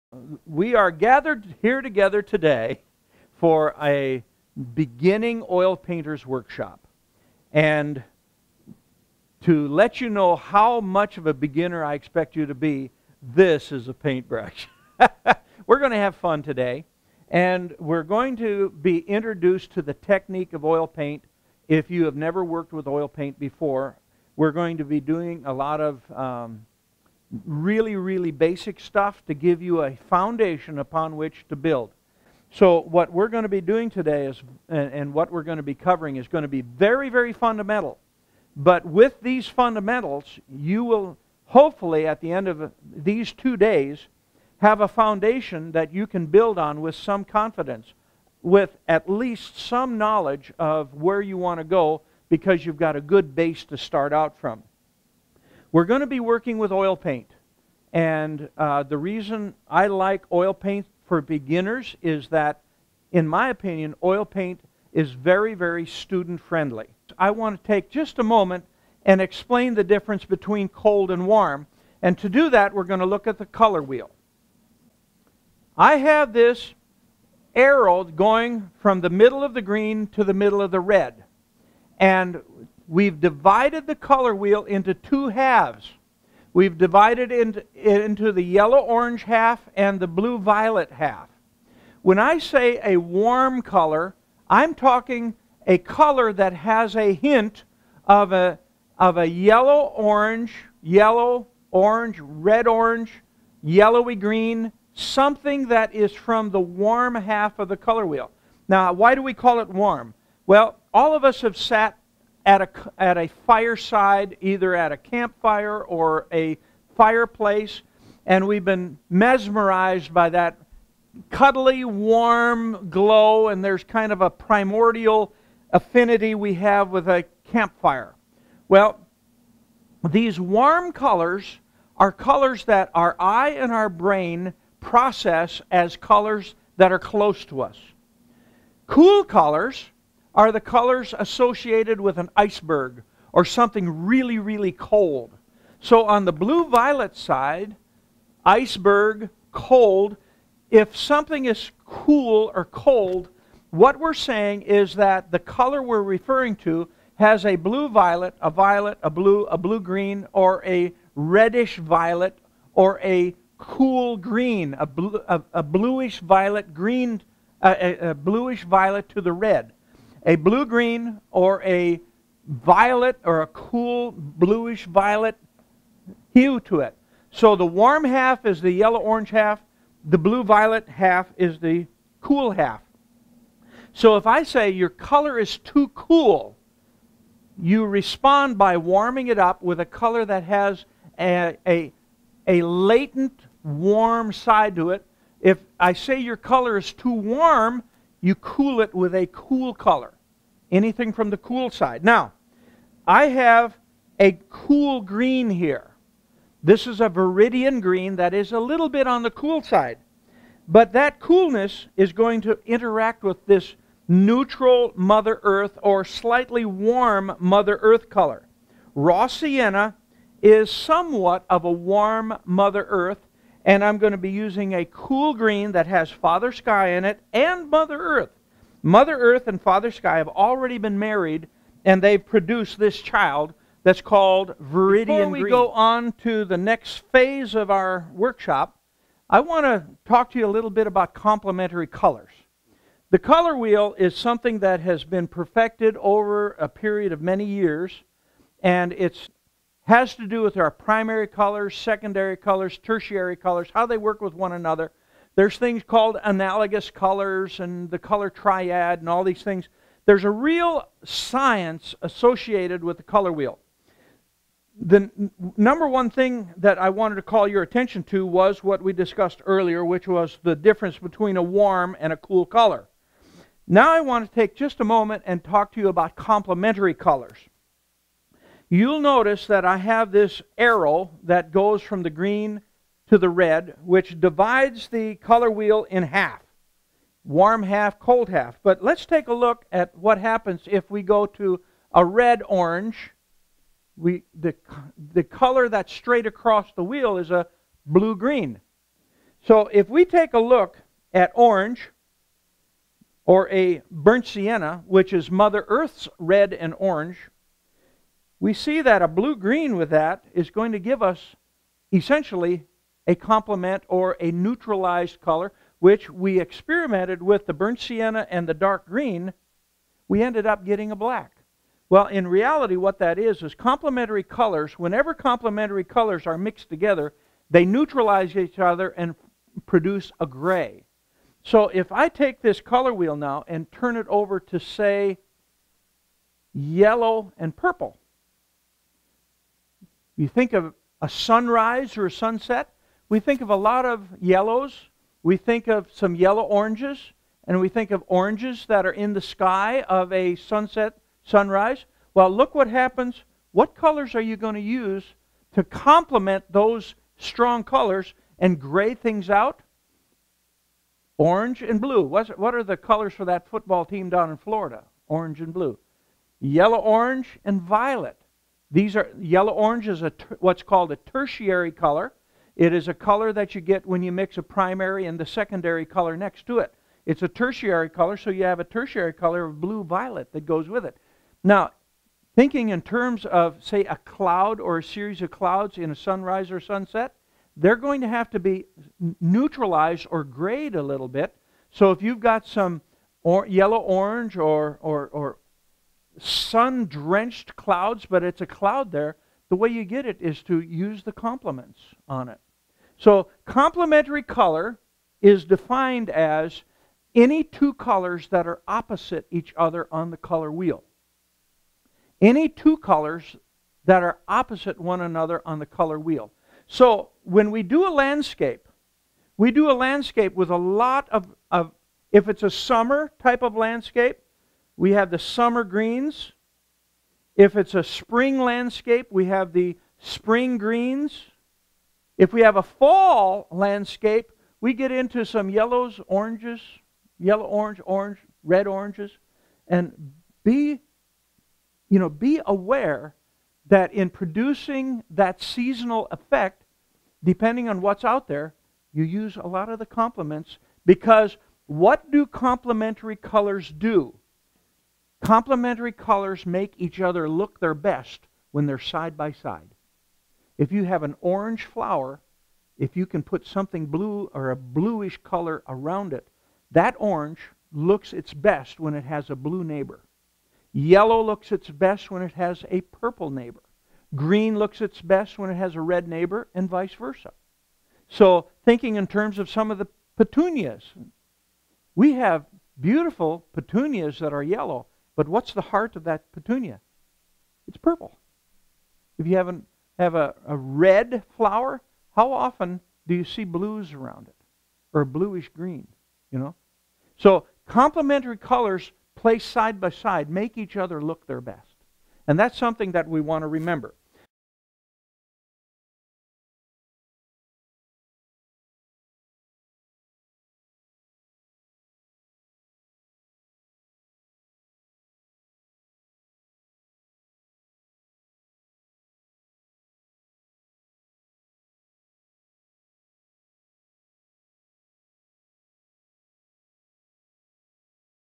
Beginning Oil Painting by Jon Houglum Video #1
Demo on how to prepare canvas, how to use the painter’s palette, how to mix colors, the tenting power of colors, the color wheel, basic drawing and painting techniques. I demonstrate how to block in the sky strata and the mountains in the background and how to blend the paints to soften the edges.  My lesson on how paint a tree is the simplest way I know to paint a tree. I demonstrate how to use lights and darks to get visual interest. Instructions are available on DVDs and USB Flash Drives on our